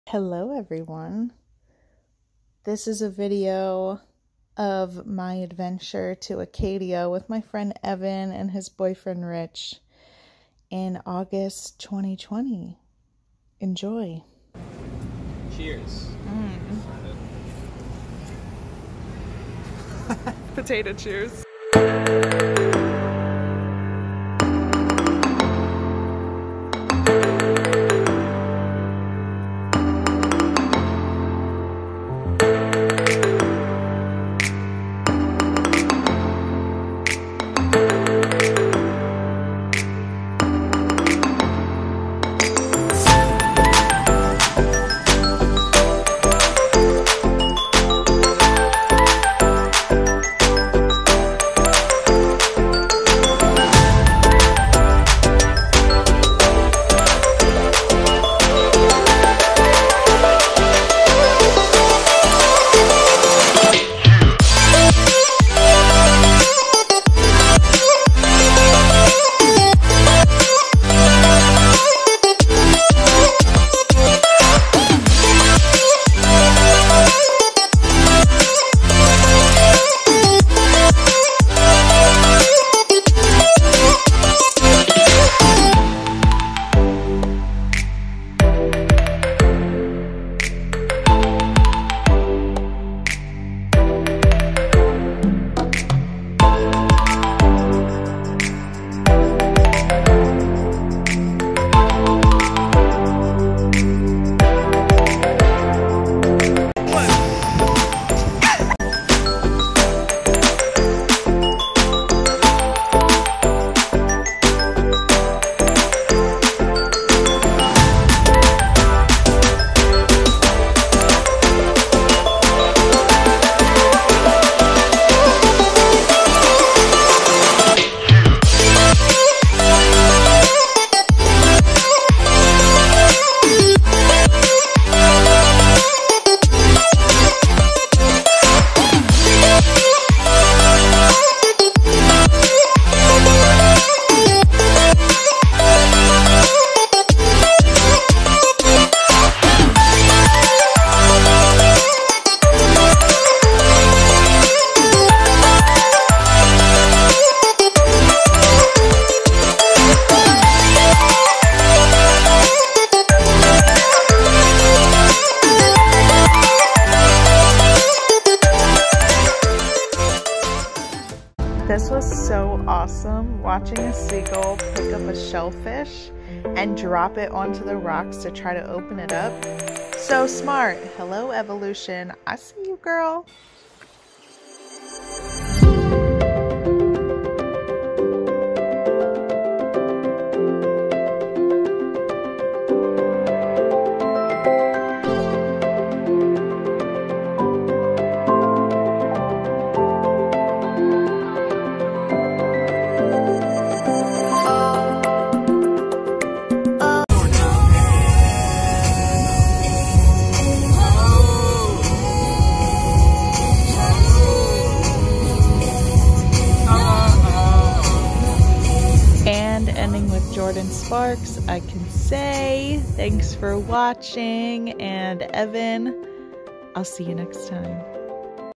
Acadia National Park 2020
Follow myself, Evan, and Rich on a quick trip up to Acadia National Park. Yes, this was a year ago... Finally edited the videos together in Sept. 2021... whatever.

Musician: @iksonmusic